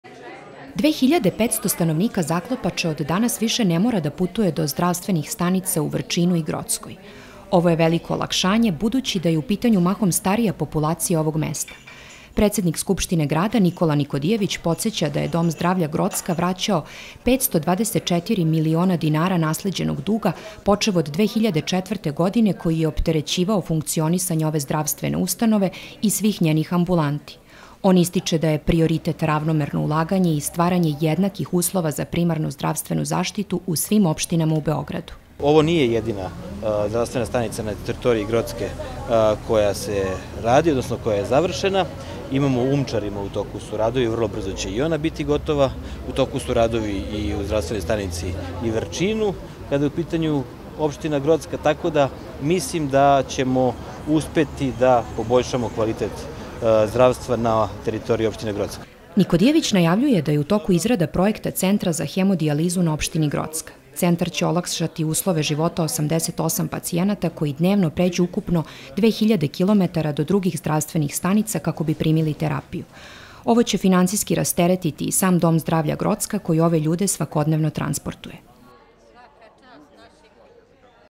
Rekonstruisana zdravstvena stanica u Zaklopači
Predsednik Skupštine grada Nikola Nikodijević rekao je na događaju da je naselje Zaklopača posle rekonstrukcije dobilo optimalnu zdravstvenu stanicu u kojoj osnovnu zdravstvenu zaštitu može da dobije 2.500 ljudi.